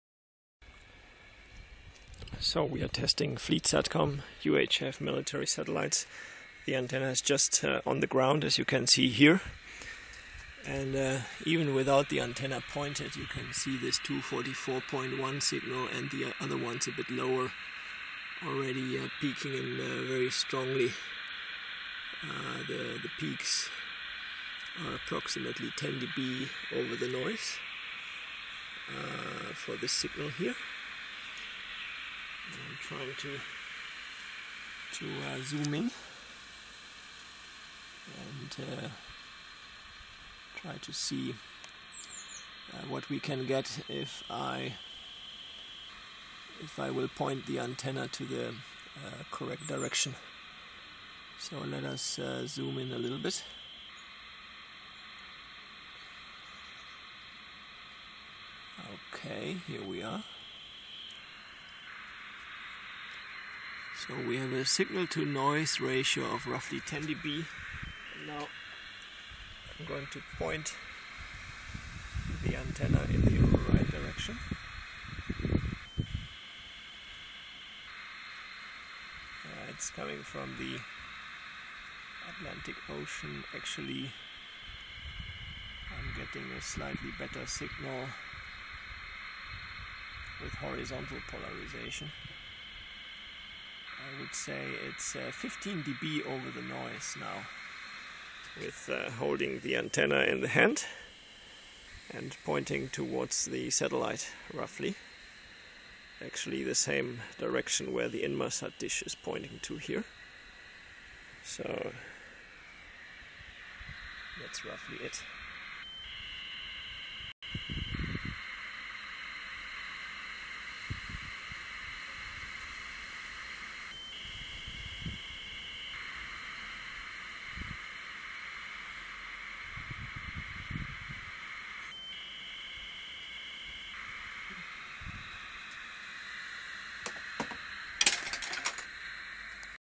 UHF MIL SATCOM with 6el Yagi and cheap RTL SDR RX
Testing a homebrew 6el Yagi with no preamp directly connected via 2m coax cable to a DVB-T dongle used as cheap SDR.
Nice signal strengths from UHF military satellites. These data streams are around 244 MHz. There are various downlink signals in the 240-270 MHz range.

These are only used as test signals since they are highly encrypted and cannot be decoded, of course...